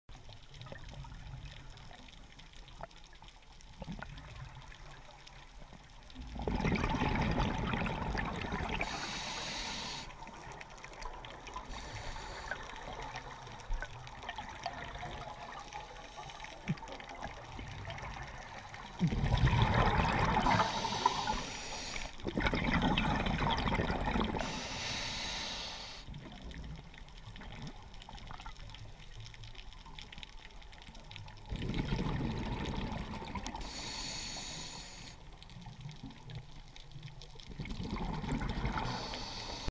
Neutral bouyency needed ??
One diver is neutral bouyent, one is not! If you find yourself you must swim and use hands, you sink a little, you need to add air in bcd jacket. Your breathing will be better and your dive will be longer. This skill is necessary for deep diving!